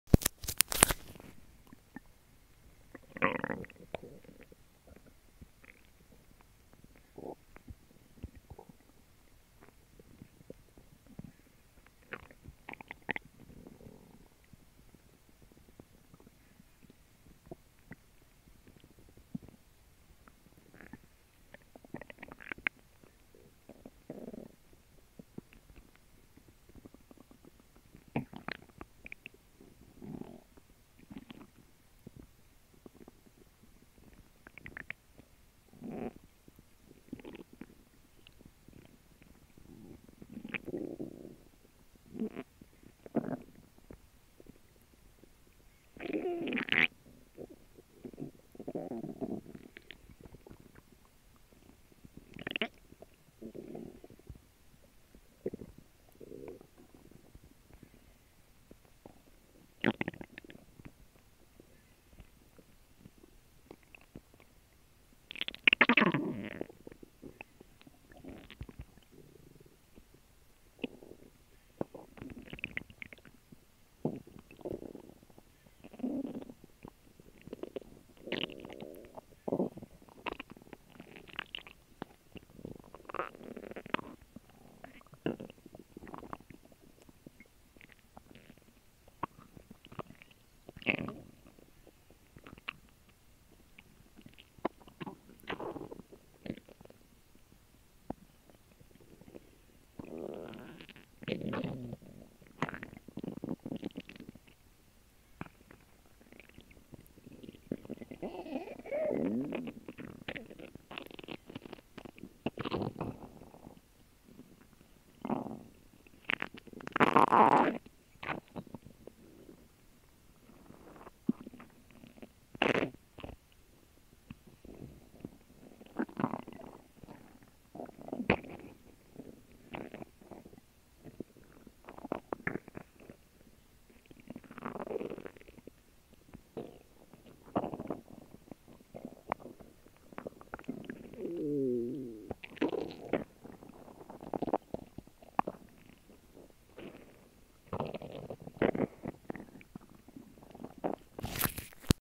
Noisy Digestion
I had a grilled cheese sandwich and water for lunch and my stomach is seriously working on them.